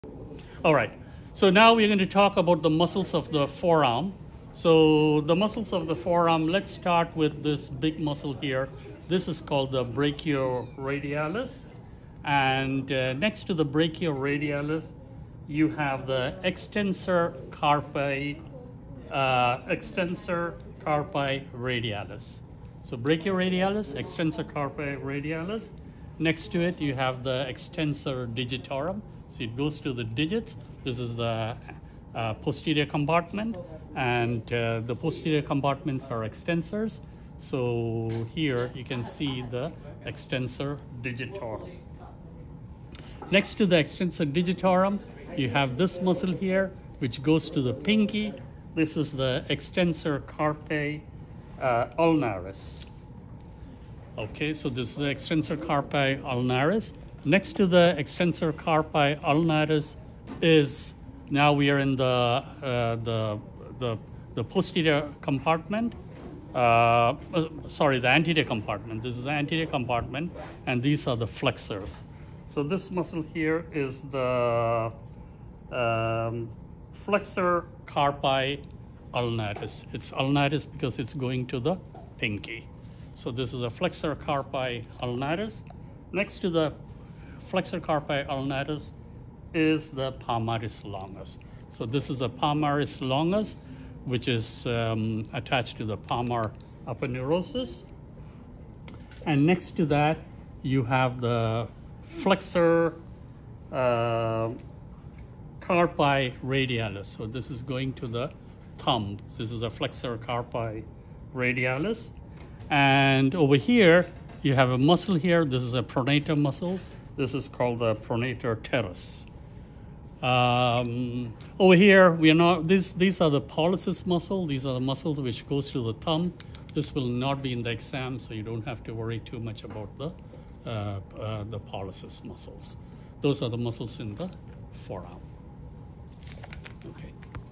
Muscles of the Forearm by Dr.K
In this presentation - Dr.Kuriakose explains the muscles of the forearm